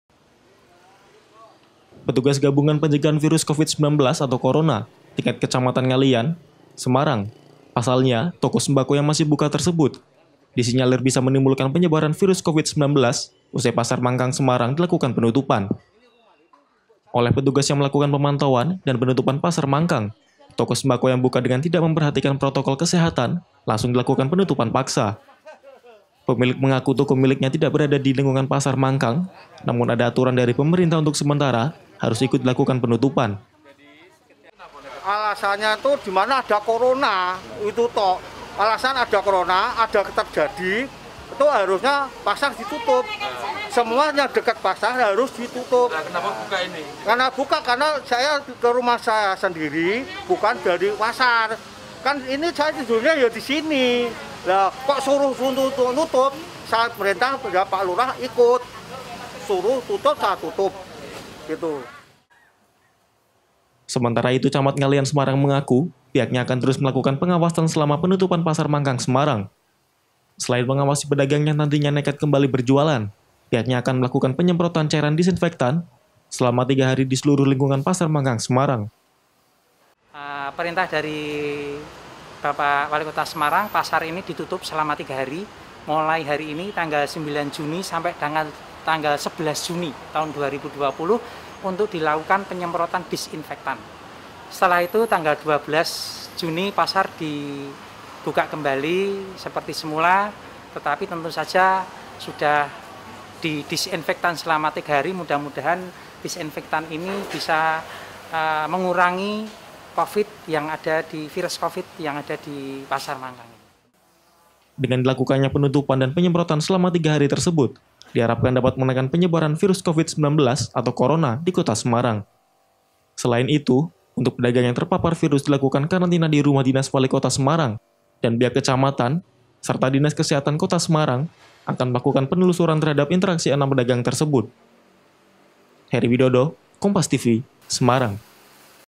Nekat Buka Petugas Tutup Paksa Toko Sembako
SEMARANG, KOMPAS.TV - Petugas gabungan pencegahan virus Covid-19 atau corona tingkat Kecamatan Ngaliyan, Semarang, Selasa pagi (09/06/2020) ) menutup paksa toko sembako milik warga yang bersebalahan dengan pasar mangkang semarang. Pasalnya, toko sembako yang maish buka tersebut, disinyalir bisa menimbulkan penyebaran virus covid 19 usai pasar mangkang semarang dilakukan penutupan.

Oleh petugas yang melakukan pemantuan dan penutupan pasar mangkang. Toko sembako yang buka dengan tidak memperhatikan protokol kesehatan langsung dilakukan penutupan paksa.

Sementara itu camat ngaliyan semarang mengaku, pihaknya akan terus melakukan pengawasan selama penutupan pasar mangkang semarang. Selain mengawasi pedagang yang nantinya nekat kembali berjualan, pihaknya akan melakukan penyemprotan cairan disinfektan selama tiga di seluruh lingkungan pasar mangkang semarang.

Dengan dilakukannya penutupan dan penyemprotan selama tuga hari tersebut, diharapkan dapat menekan penyebaran virus covid 19 atau korona di kota semarang. Selian itu, untuk pedagang yang terpapar virus dilakukan karantina di rumah dinas walikota semarang, dan pihak kecamatan serta dinas kesehatan kota semarang akan melakukan penelurusan terhadap interaksi enam pedagang tersebut.